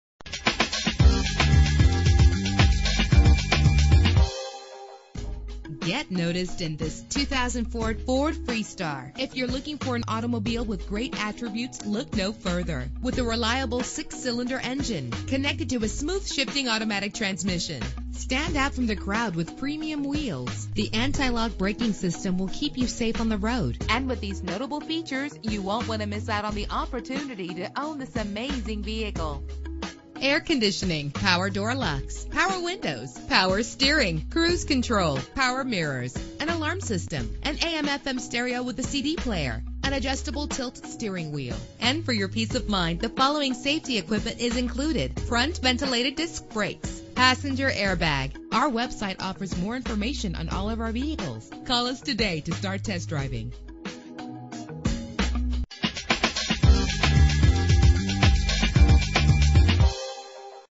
2004 Ford Freestar - Jackson Heights NY
Year: 2004
Make: Ford
Model: Freestar
Trim: SE
Engine: 3.9 liter 6 cylinder 12 valve
Transmission: 4-Speed Automatic
Color: Silver
Mileage: 77500
Address:
7415 Northern Blvd